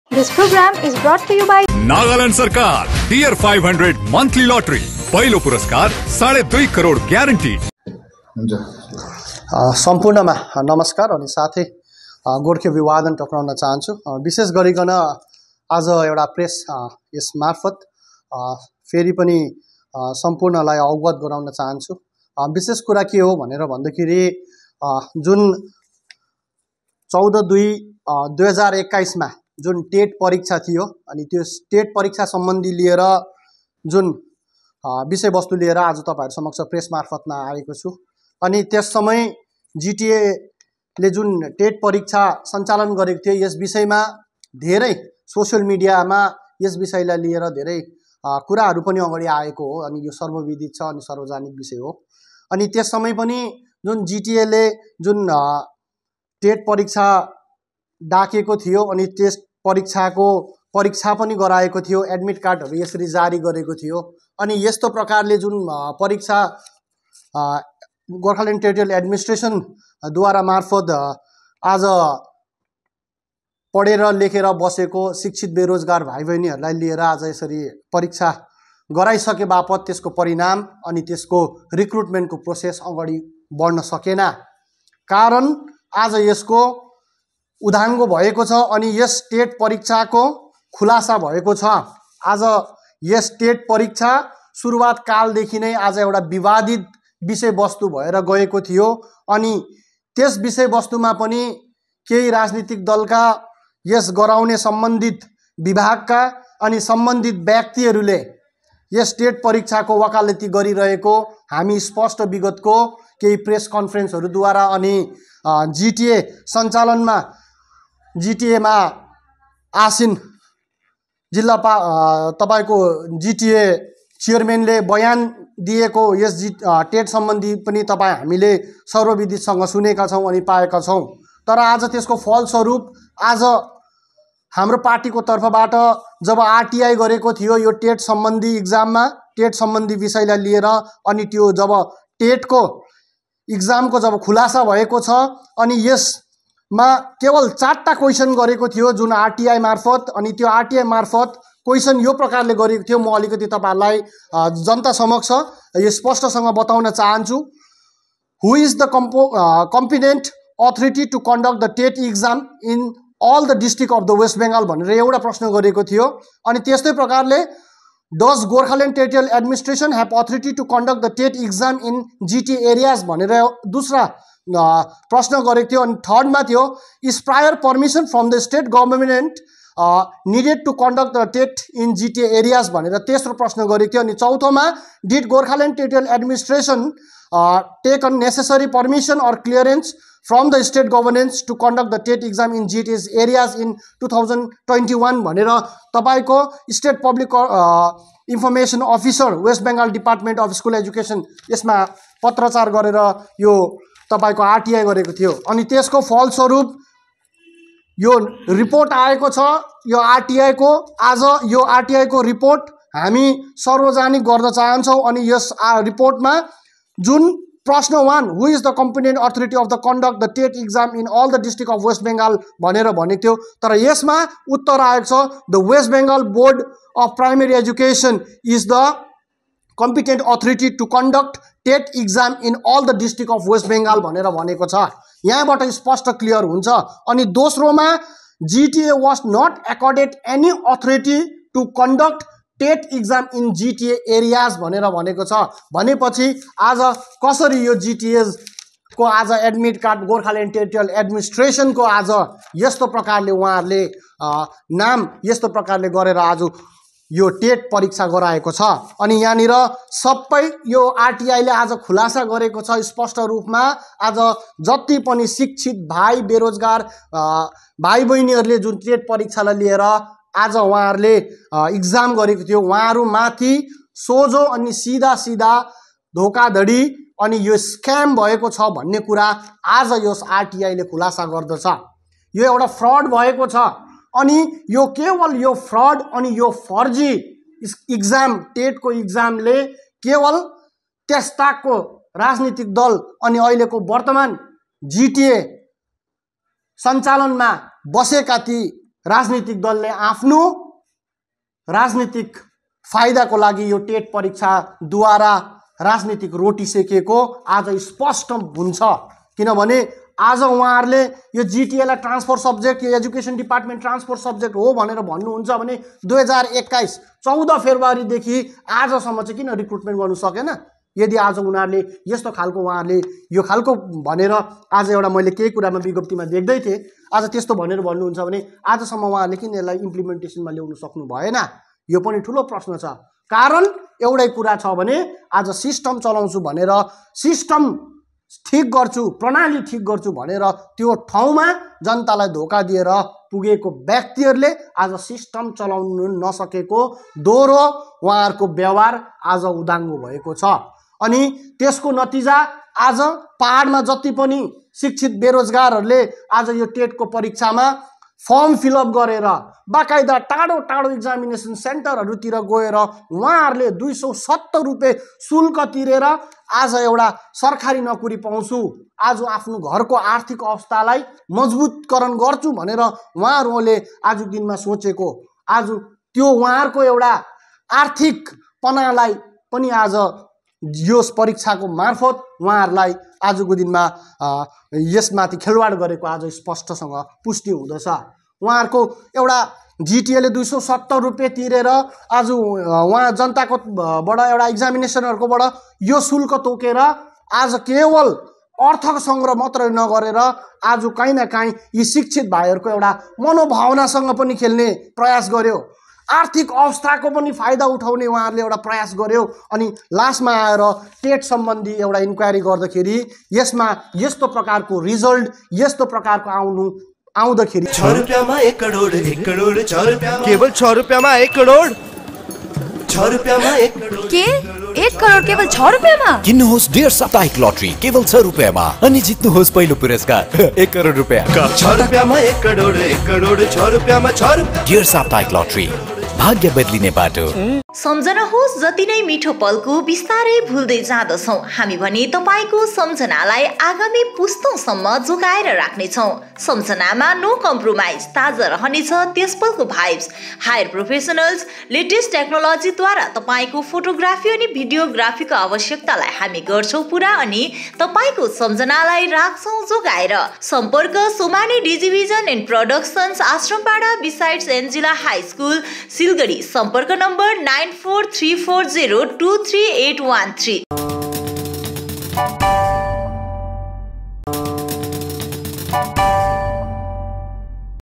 फर्जी टेट परीक्षा गराएर जि टि एले शिक्षत्रित युवाहरुको भविष्य माथी खेलवाड गरेको बताउदै यसको निम्ति
फर्जी टेट परीक्षा गराएर जि टि एले शिक्षत्रित युवाहरुको भविष्य माथी खेलवाड गरेको बताउदै यसको निम्ति अनित थापाले माफि माग्नु पर्ने नत्र जि टि ए को चौकी छोड्नुपर्ने श्याम गौतमको माग